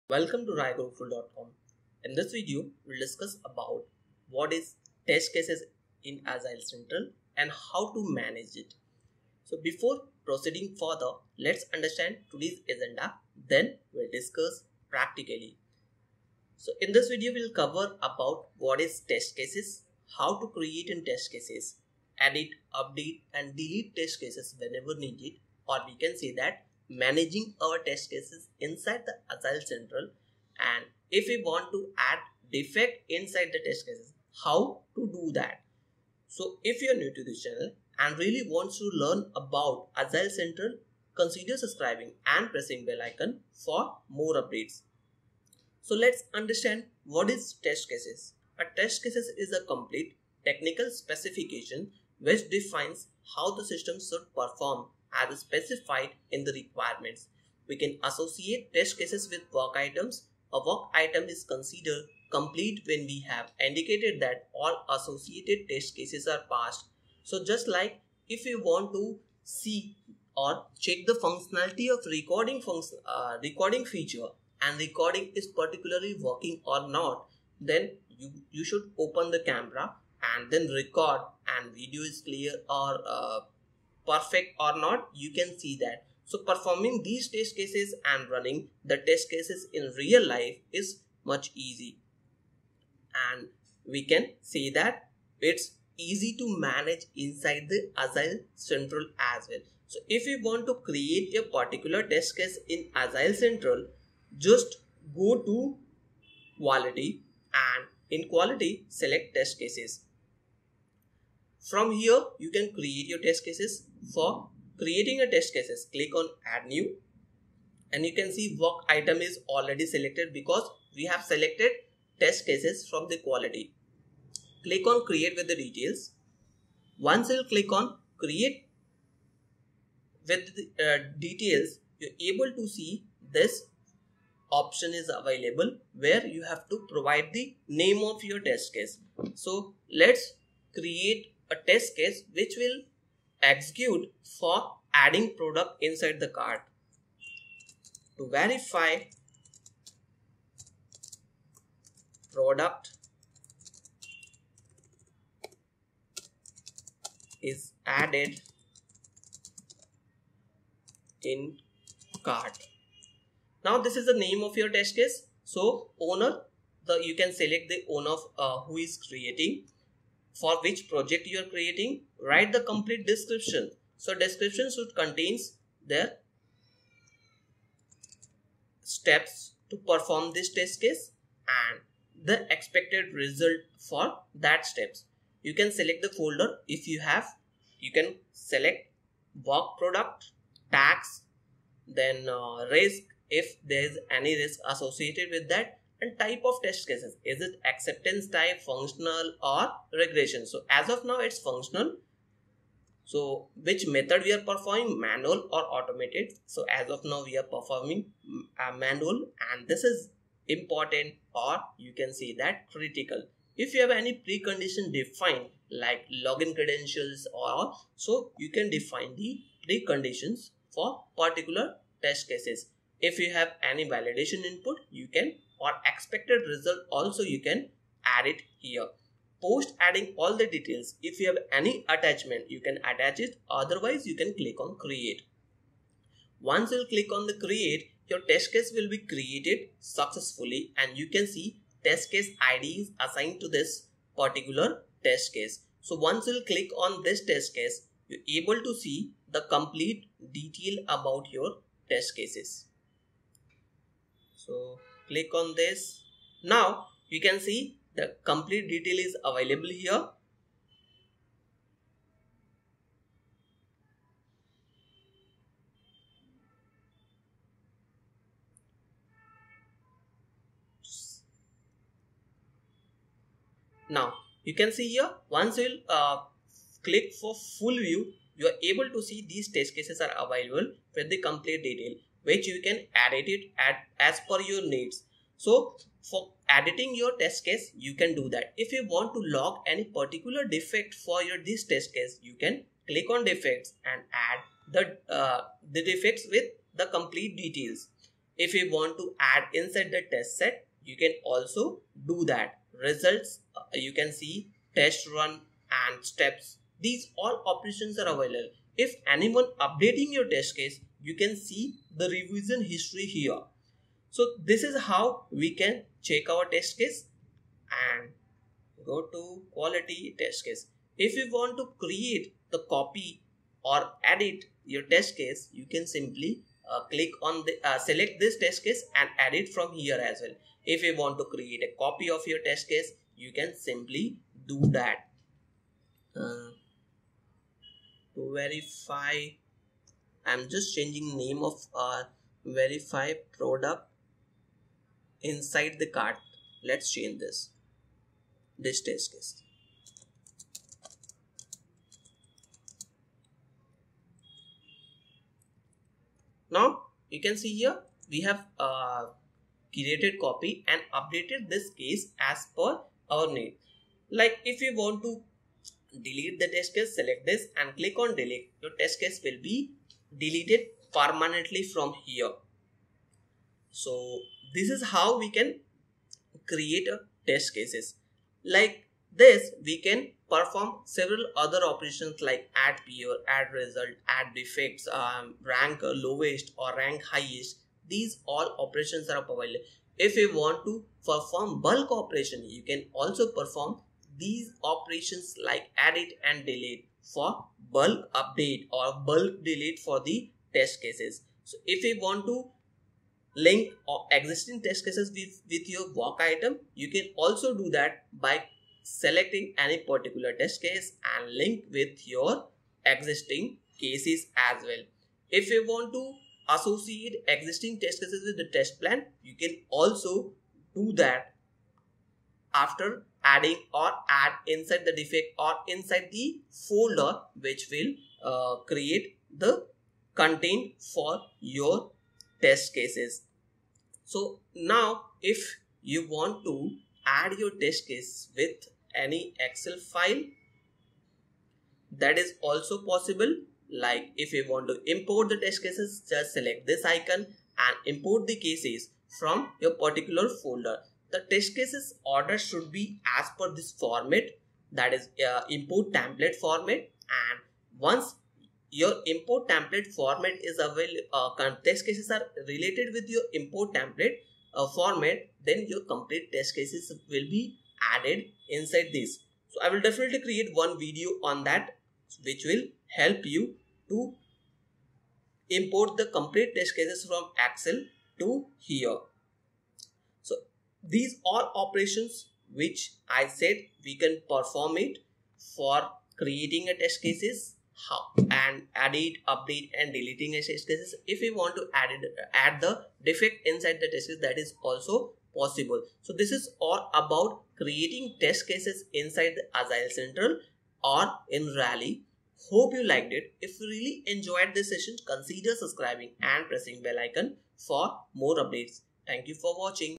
Creating and Managing Test cases in Rally | Create Test case in Agile Central | Rally Tutorial
A test case is a complete technical specification which defines how the system should perform as specified in the requirements. You can associate test cases with work items. A work item is considered complete when you have indicated that all associated test cases pass.

Creating Test Cases
To save time and have easier tracking, Rally recommends creating test cases directly from an associated work item. When you create a test case from a work item, Rally creates an automatic association between the work item and the test case. You can then view the association in the detail editor of either the work item or the test case. When you create a standalone test case, you must manually associate the test case to a work item.

Watch complete video for more detail.